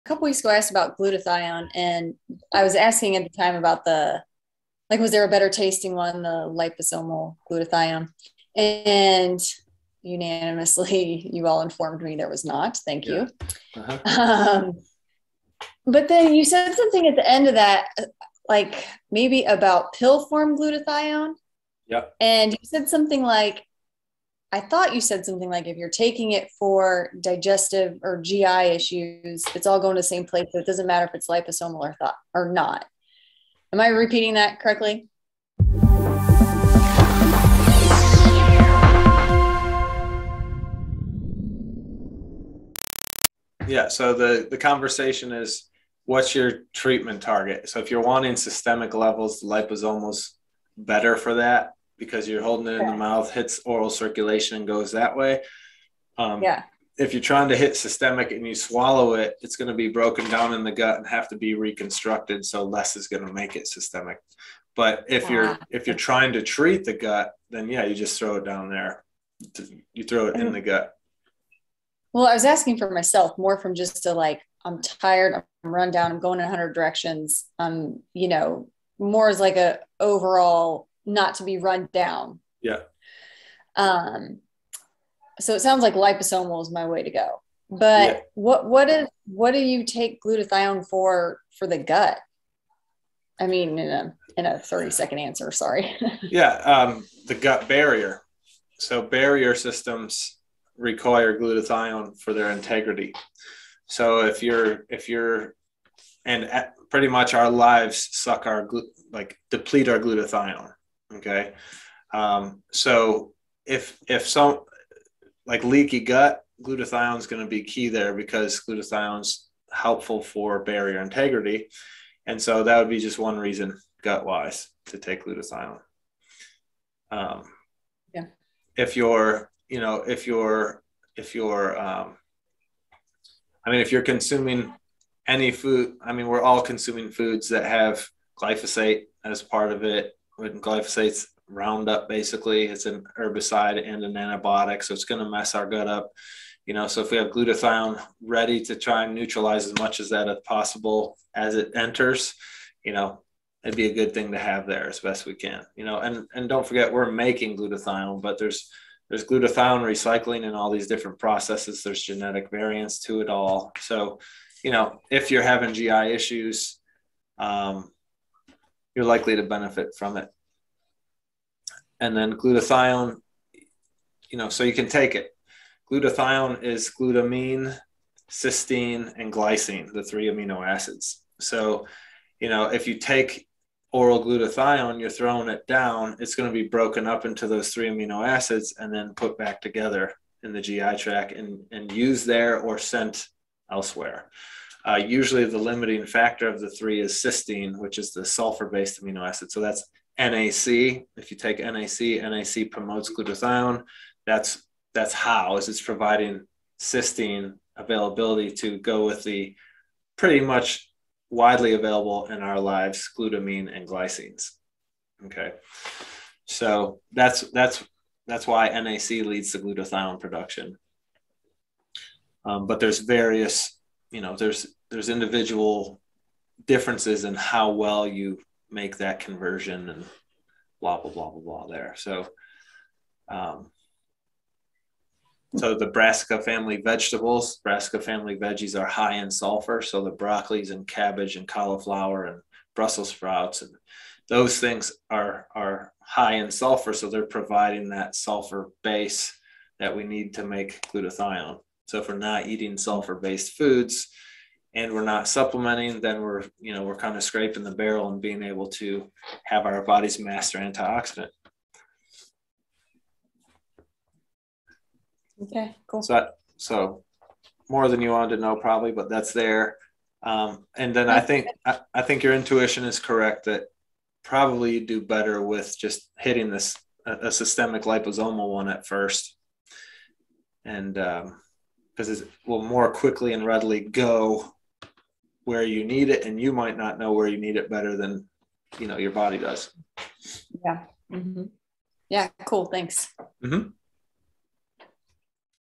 Glutathione - What is better, capsule or liposomal form? Why?
The delivery method of glutathione matters clinically based on what the target tissue is that you are trying to impact.

When is a capsule the best choice versus a liposomal form?

Why does it matter?